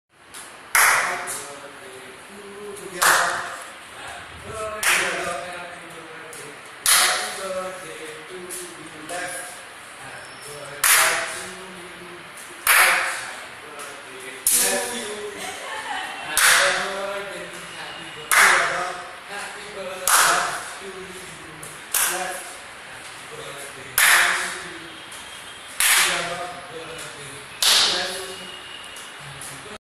A New Code NLP Game ( A glimpse of it) | Abhay Thakkar - New Code NLP Trainer from India
New Code NLP games are designed to unleash a high performance state in a person by activating the unconscious mind by playing games.

In New Code NLP , we work at the level of state.
As John Grinder (co-creator of NLP & New Code NLP) would say - " Problem is not the problem, the state in which you enter the problem is what needs to be addressed"